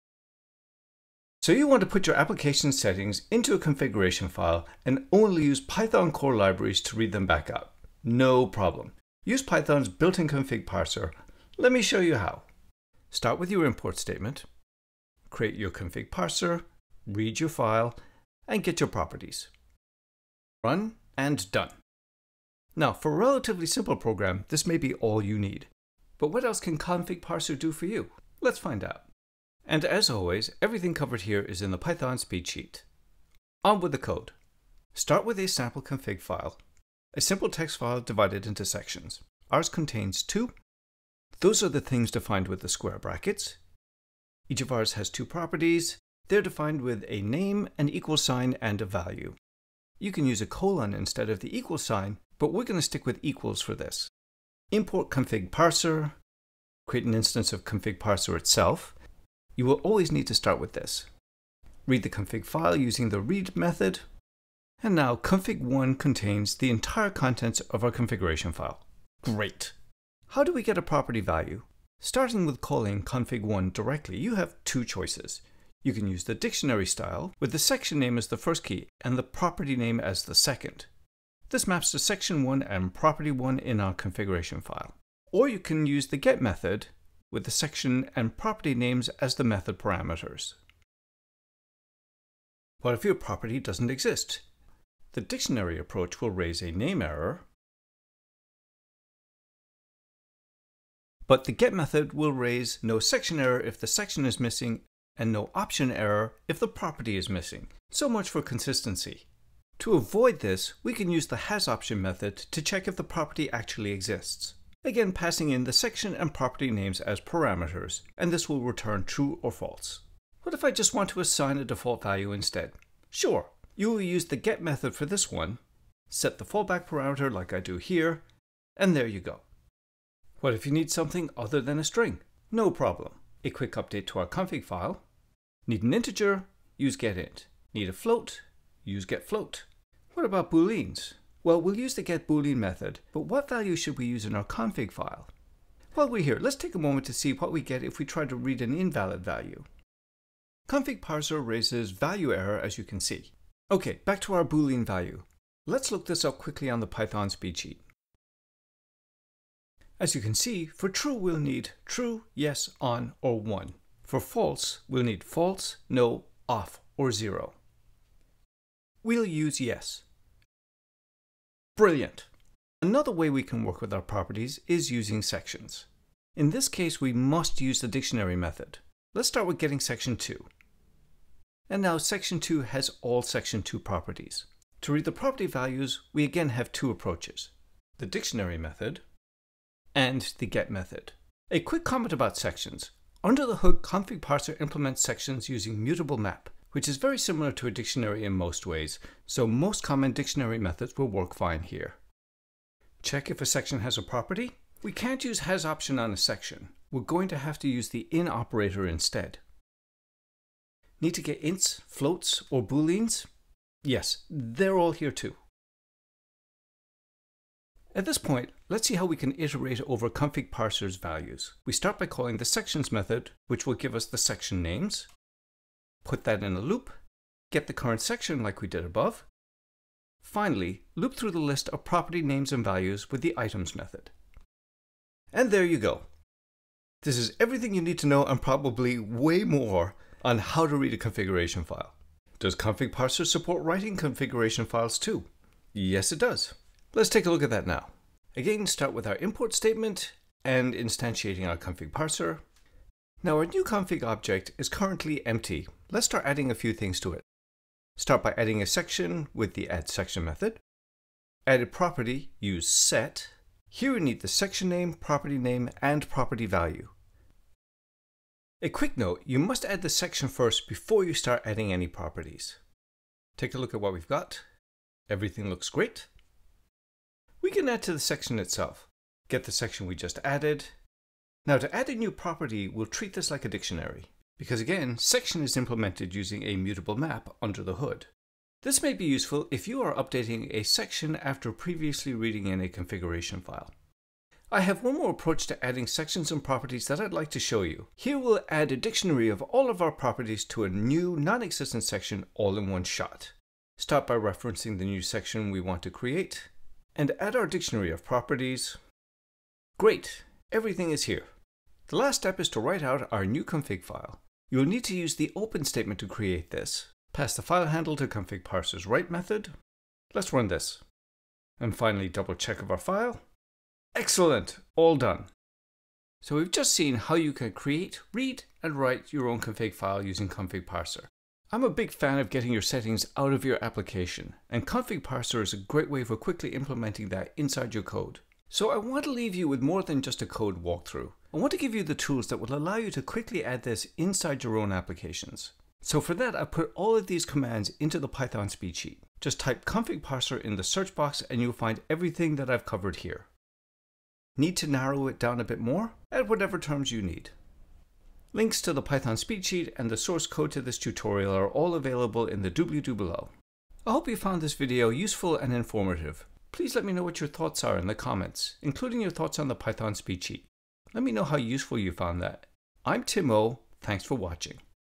Add a Quick Config File With Python's ConfigParser
Python's ConfigParser:

Use ConfigParser to quickly add configuration files to your Python projects.
Add it faster and easier again with the Python SpeedSheet.